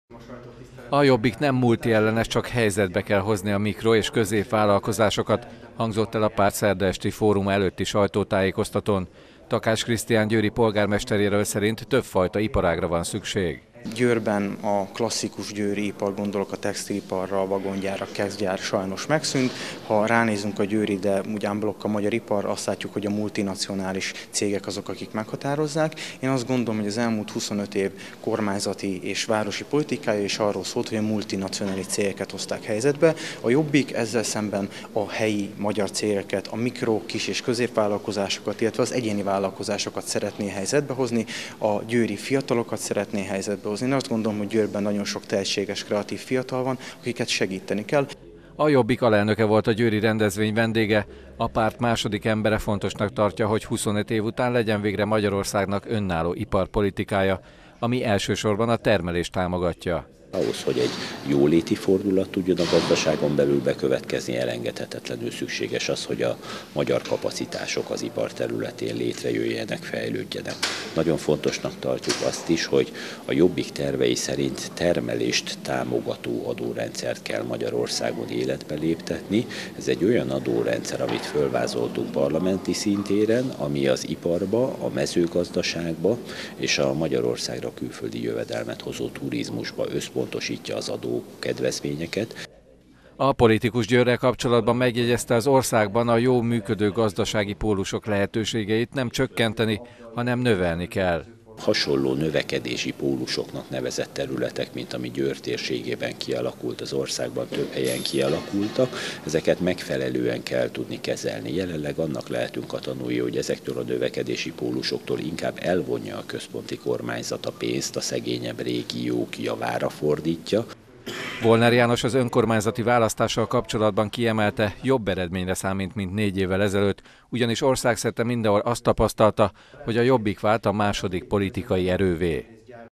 Beszűkült a gazdaság
Győrben eltűntek a klasszikus iparágak és többségében csak a járműgyártáshoz kapcsolódó vállatok maradtak –jelentette ki Takács Krisztián, a Jobbik polgármesterjelöltje csütörtök esti gazdasági fórumán. A politikus ezen változtatna a mikro és középvállalatok megerősítésével. Volner János, a Jobbik alelnöke pedig a termelést támogató adórendszer bevezetését tartja fontosnak.